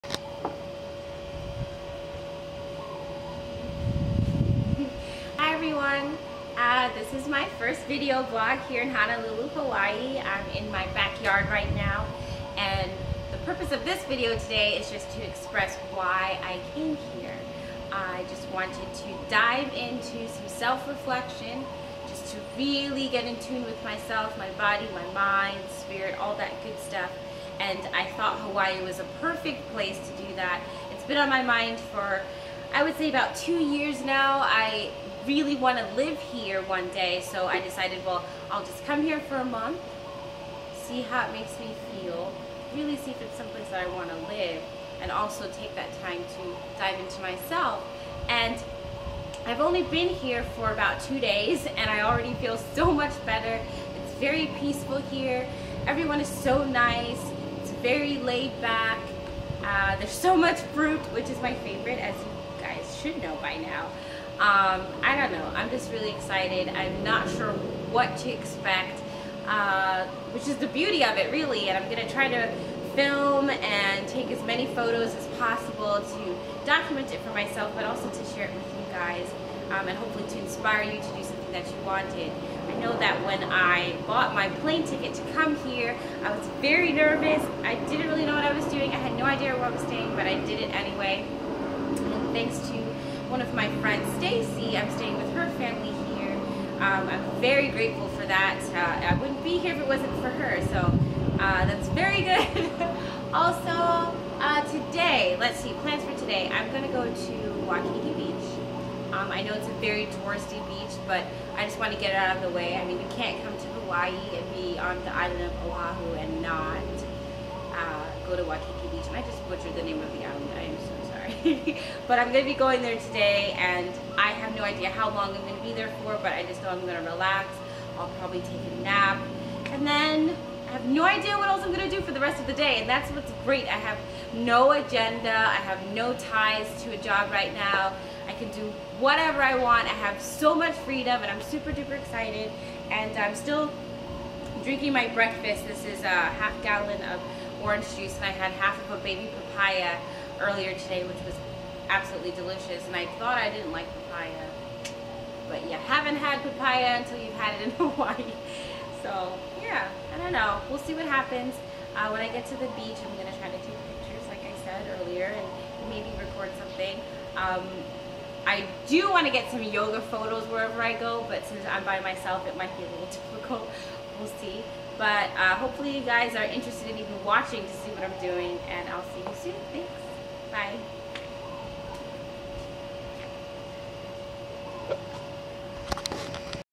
My Hawaii Happymoon!
Simply sharing my 30 day journey in Hawaii with you through vlogs! :)